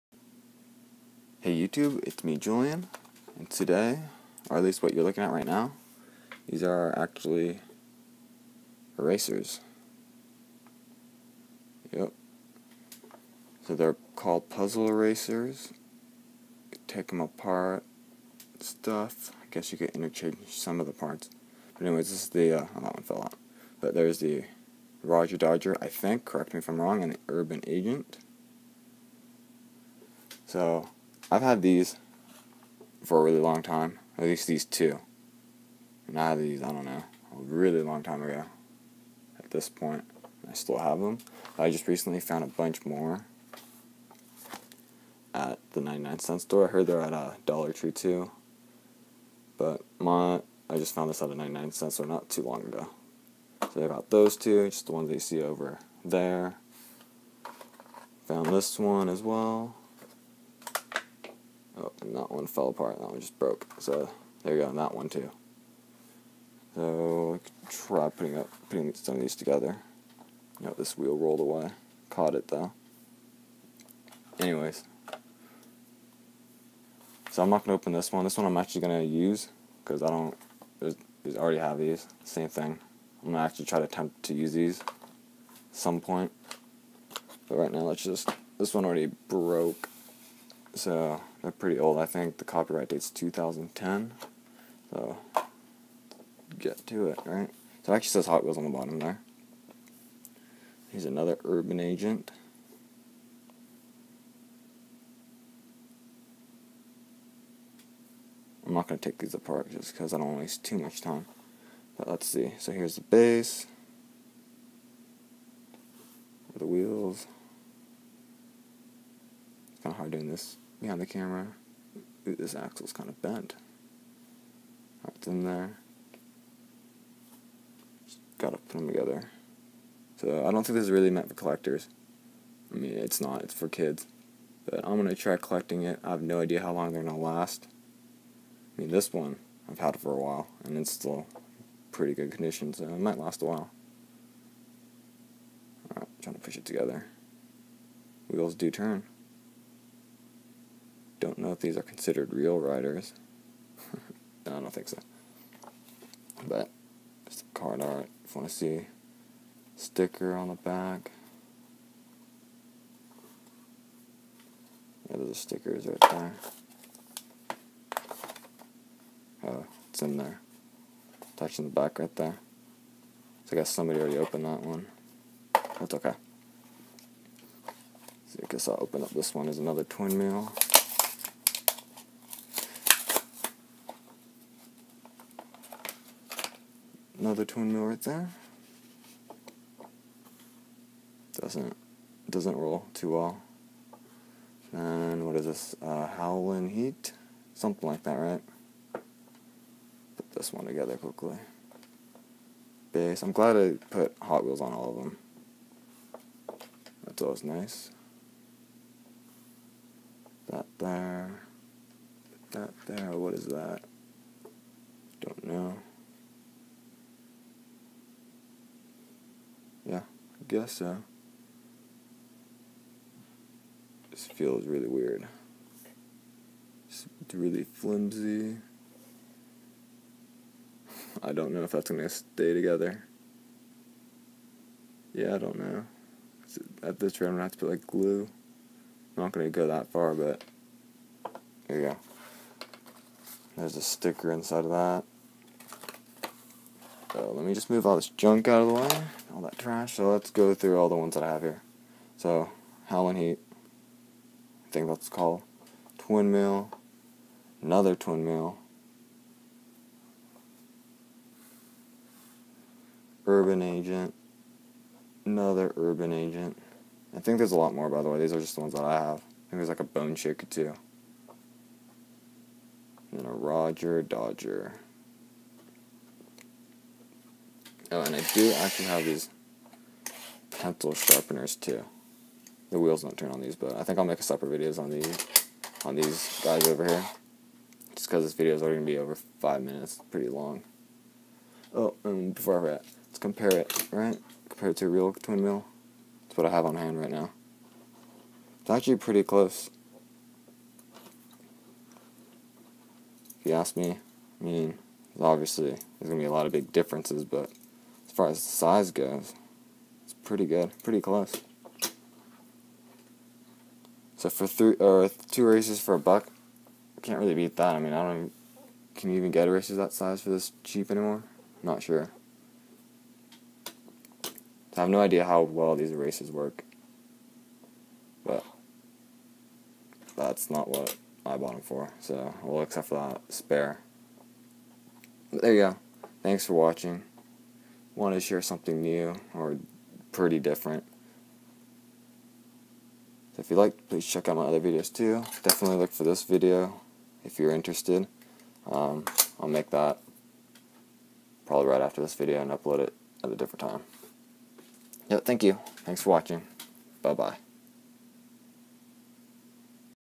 Hot Wheels Erasers! (Puzzle Erasers / Crazerasers / Eraser Buddies / 1:64 Scale Eraser Castings)
Collectible Hot Wheels erasers? Yup! Take them apart and put them back together, and erase your problems away with these cool little soft cars!

Erasers shown in this video:

Twin Mill III (Green)
Twin Mill III (Yellow-orange)
Urban Agent (Blue)
Urban Agent (White)
Howlin' Heat (Yellow-orange)
Rodger Dodger (Bright Orange)